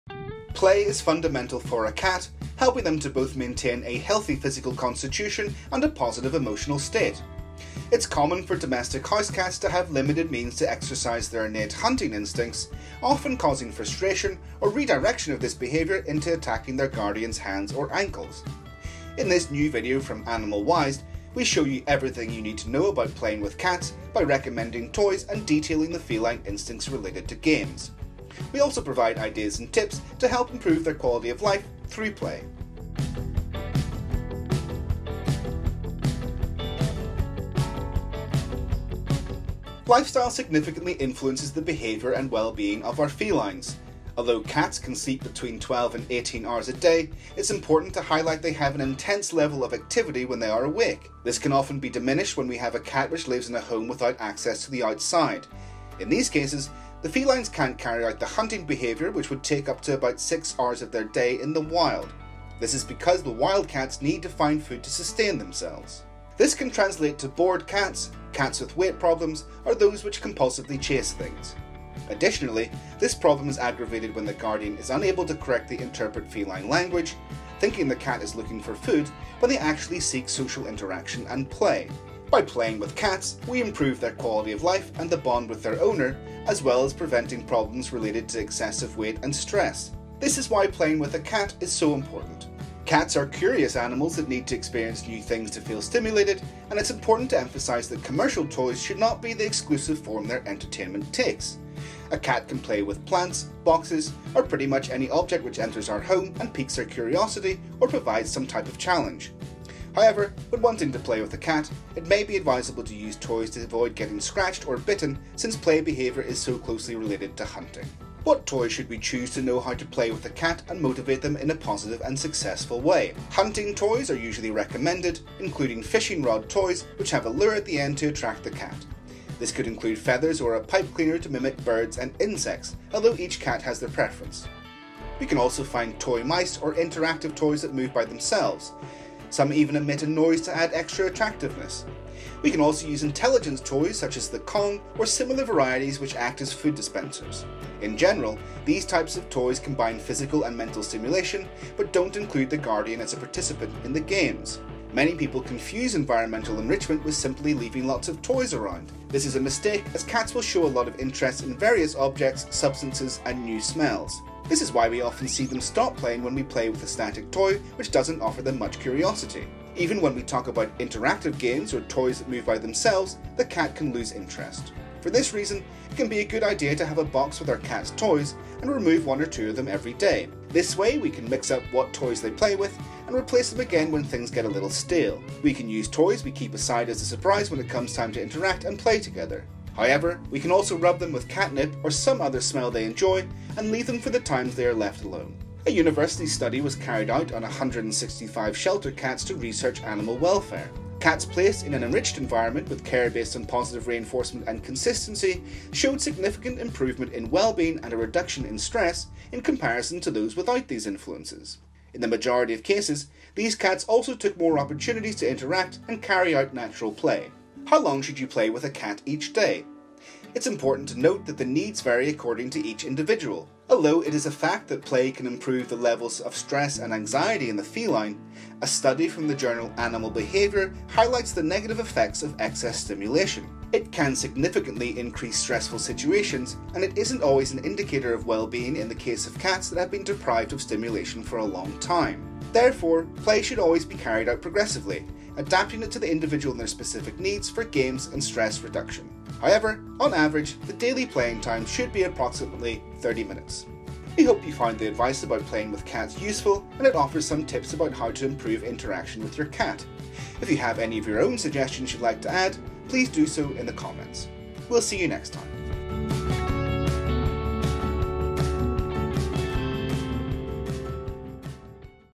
The Best Ways to PLAY with Your CAT 🐱 Toys and Games
🐱 Playing with your cat is an important responsibility for every cat guardian. In this AnimalWised, we explain how games and play help to stimulate your cat both physically and mentally, and provide practical advice on how to best play with your cat.